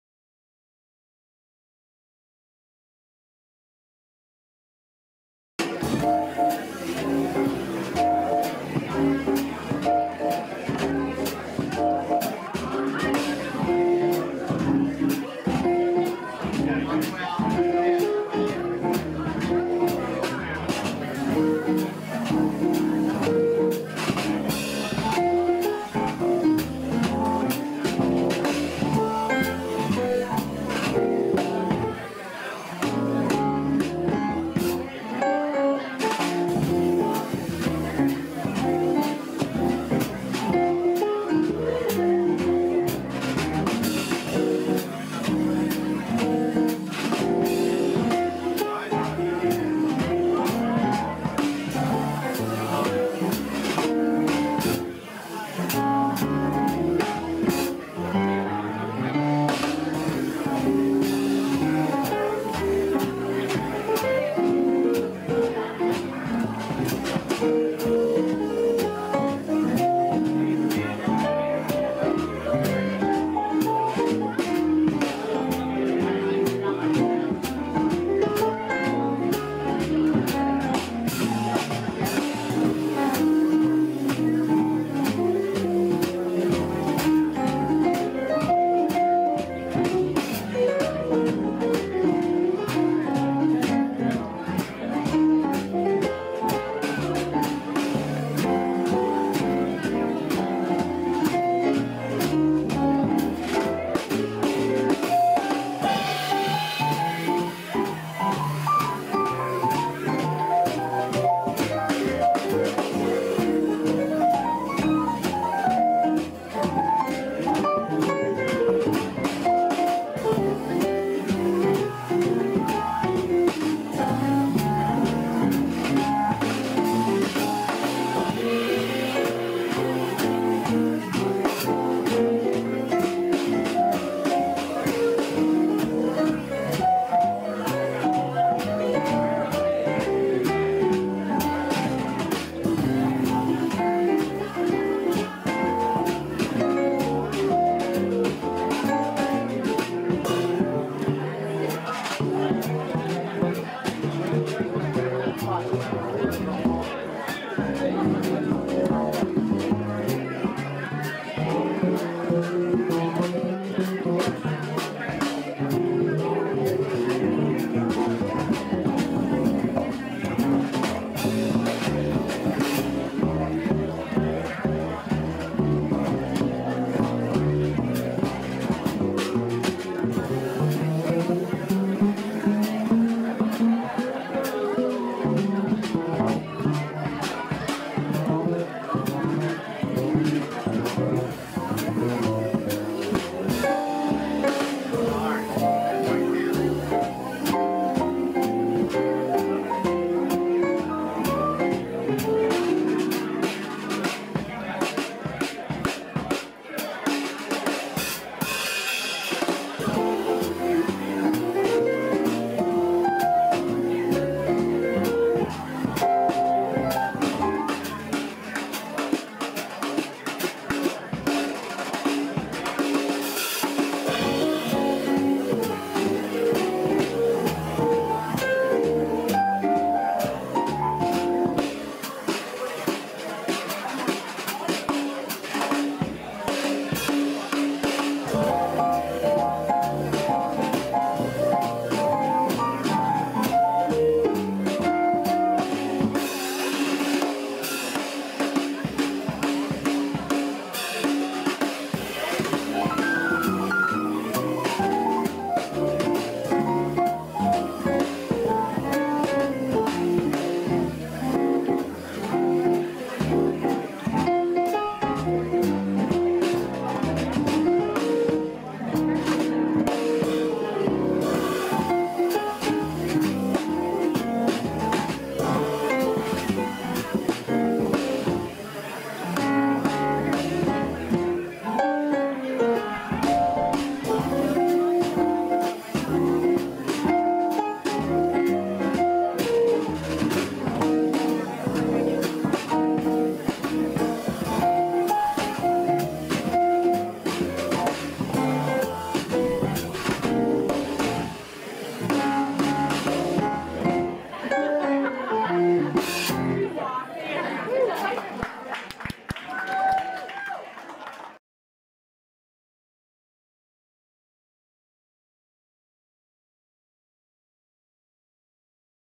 Heating it up at Bernardo Winery - San Diego California
Whitney Shay Quartet, live at The Bernardo Winery. September 6, 2015. Whitney Shay - Vocals, Jodie Hill - Bass, Michael Leroy Peed - Keyboard, Marland Waak - Drums, Christopher Hollyday - Alto Sax, Ross Rizzo Jr. – Sax